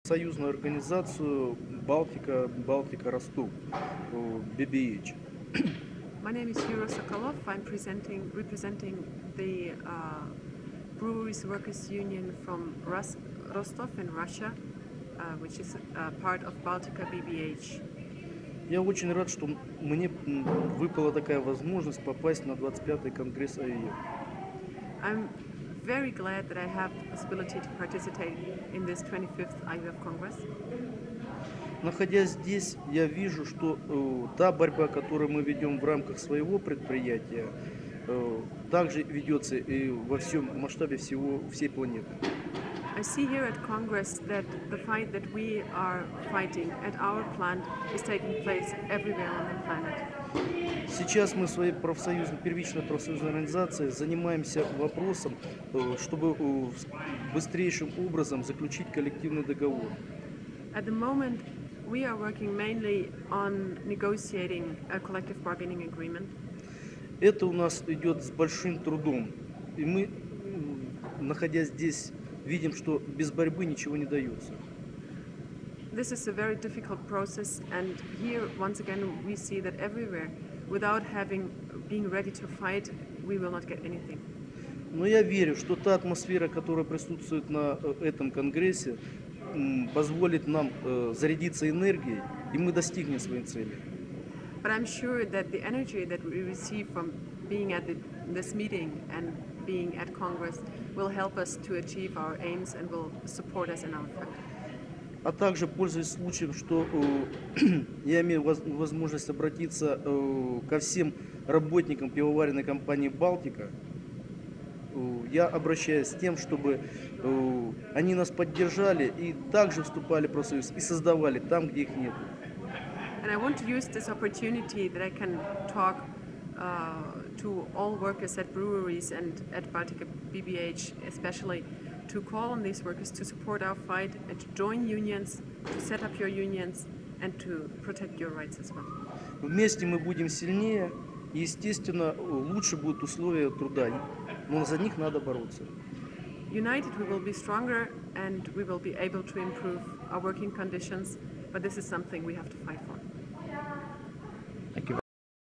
Interview with Yuri Sokolov
Yuri is from the Rostov-Baltika BBH Workers Union, in Russia. The translator is Gisela Neunhoffer.